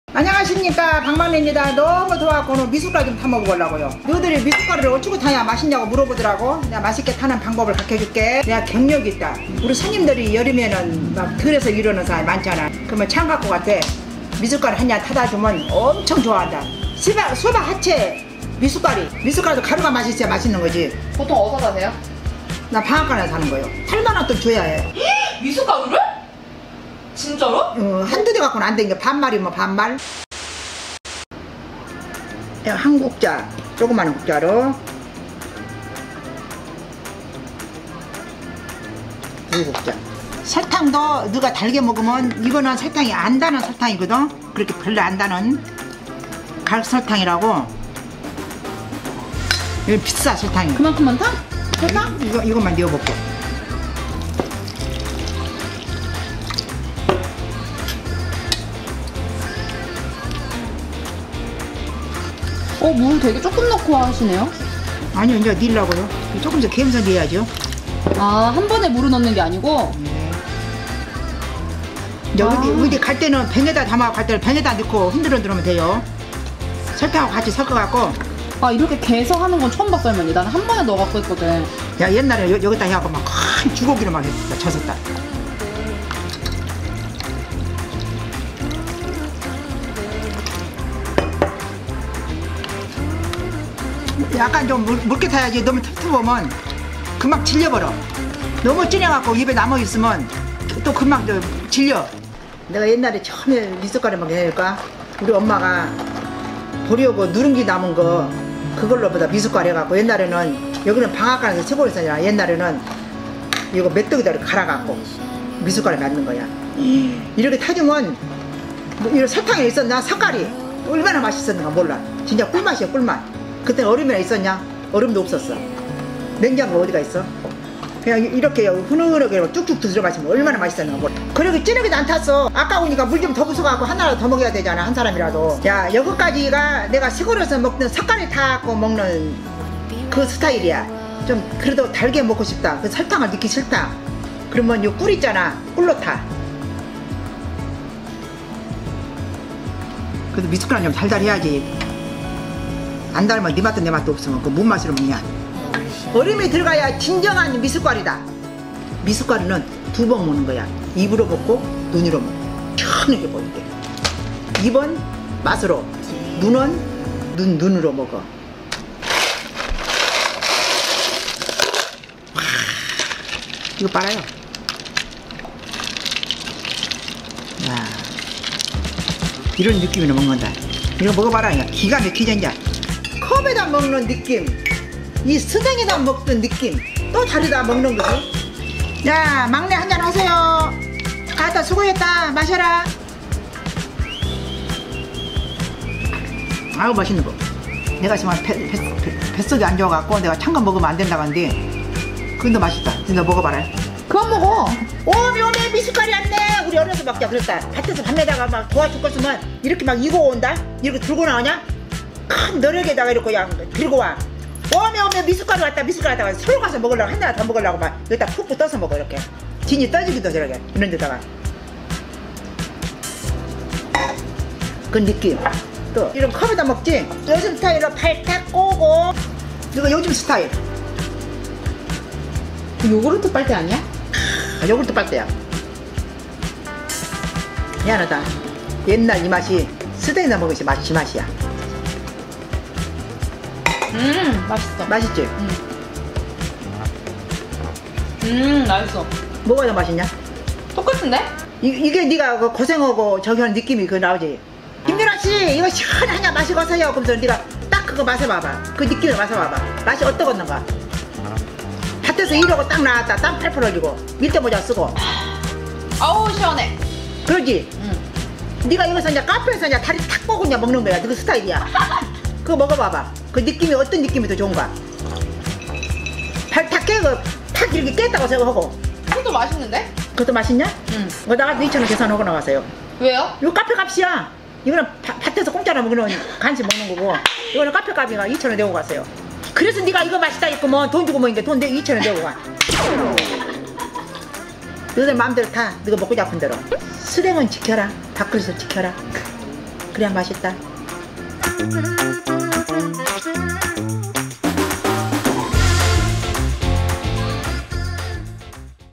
[Eng] 미숫가루 맛나게 먹는 요상한 팁
미숫가루만 8만원어치 사먹는 막례쓰의 미숫가루 팁
믹례쓰는 의외로 찐~하게 타먹지 않고 일부러 시원하게 물처럼 묽고 달게 드시는 타입!
여기에 더 맛나게 먹는 꿀팁으로.. 스뎅을 추천..ㅋㅋㅋㅋㅋㅋㅋ
♥할머니를 보고 즐거웠다면 좋아요!
♥할머니를 친구에게 소개하고 싶다면 공유하기!